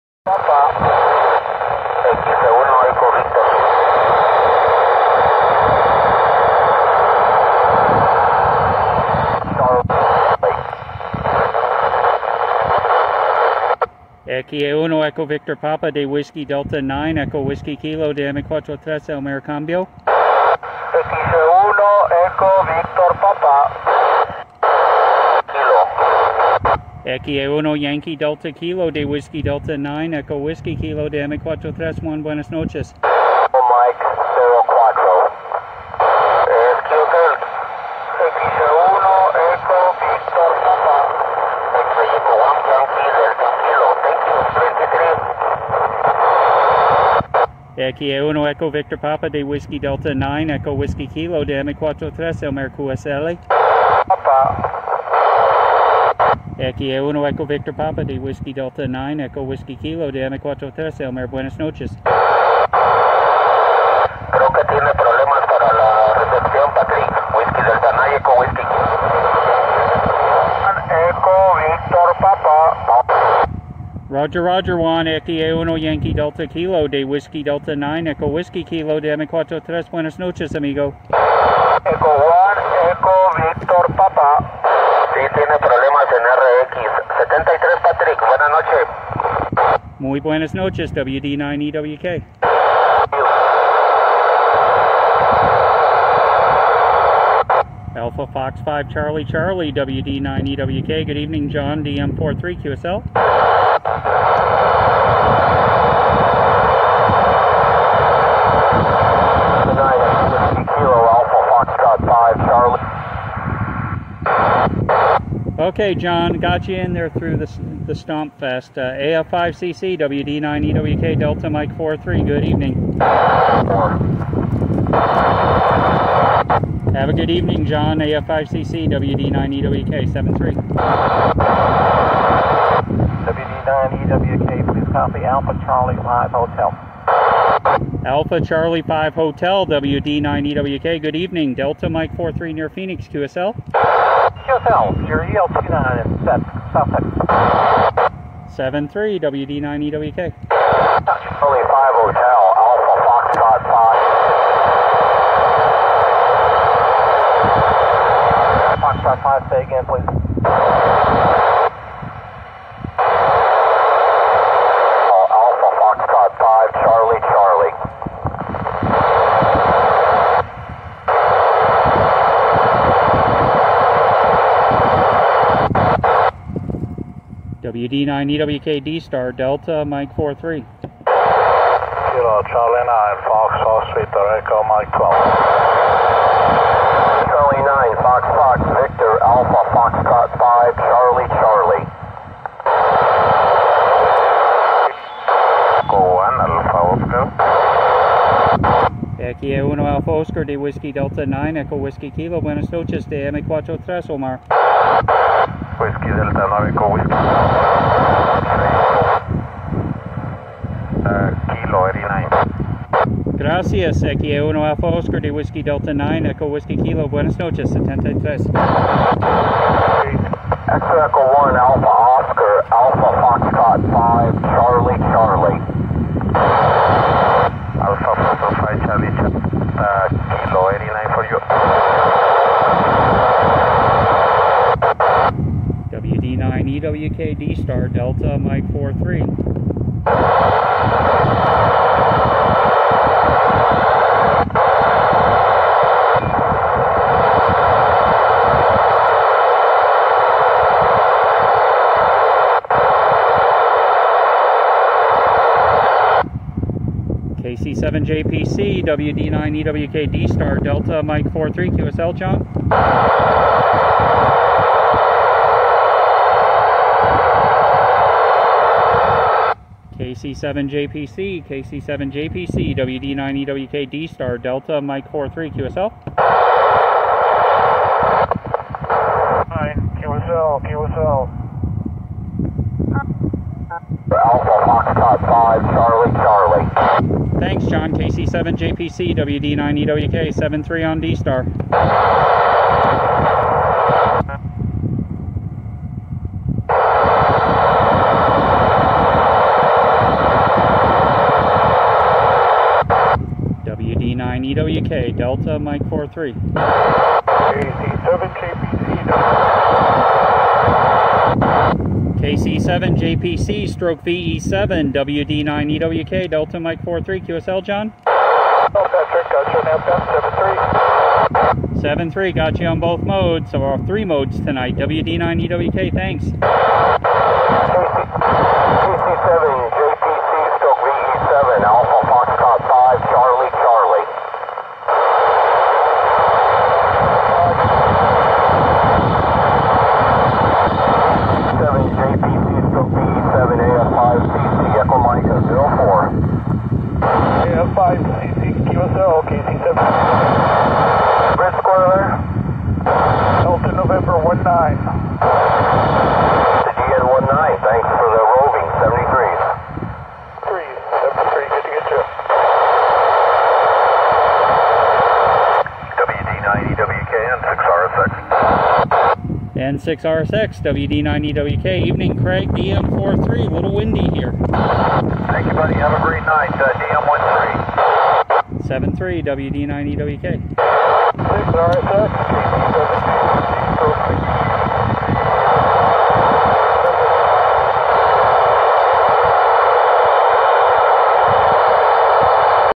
D-Star (and FM) via TEVEL-3 - 11 August 2023 @ 0437-0443 UTC
WD9EWK working the TEVEL-3 satellite using D-Star digital voice and FM from Scottsdale, Arizona (DM43), on Friday 11 August 2023 at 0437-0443 UTC. This was a 31.2-degree pass to the east of central Arizona. WD9EWK worked KC7JPC/VE7 using D-Star and FM during this pass. Other stations were heard in FM - XE1EVP, XE1YDK, XE1AO, AF5CC, AC5H, and N6RSX.

The WD9EWK station had an Icom ID-4100A 2m/70cm FM/D-Star mobile radio and an Elk Antennas handheld 2m/70cm log periodic.